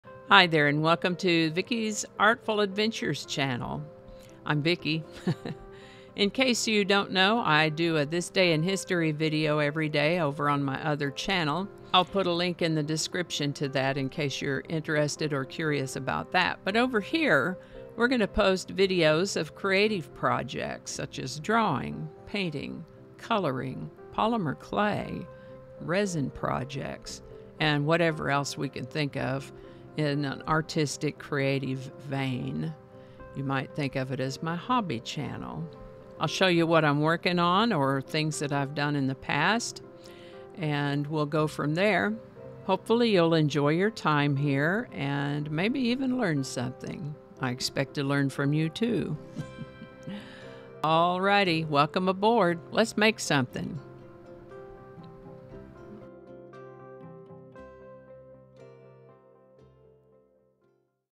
Vickis Artful Adventures Channel
Hi there and welcome to Vicki’s artful adventures channel, I'm Vicki!
Over here we're going to post videos of creative projects, such as drawing painting, coloring, Polymer Clay, resin projects, and whatever else we can think of in an artistic, creative vein. You might think of it as my hobby channel; I'll show you what I'm working on or have done in the past and we'll go from there. Hopefully you'll enjoy your time here and maybe even learn some thing. I expect to learn from you, too!
All righty, welcome aboard! Let's make something!